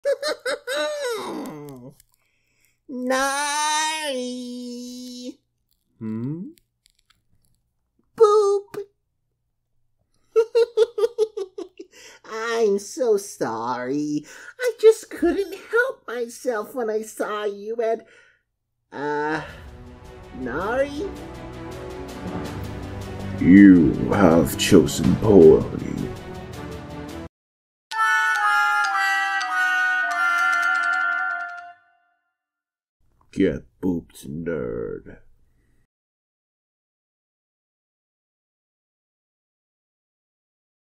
NARINDER HAS TOE BEANS! - cult of the lamb comic dub
in todays comic dub we follow the lamb who boops narinder
narinder gets vengeance by unholy toe beans

here is my throne if you want to help me improve my equipment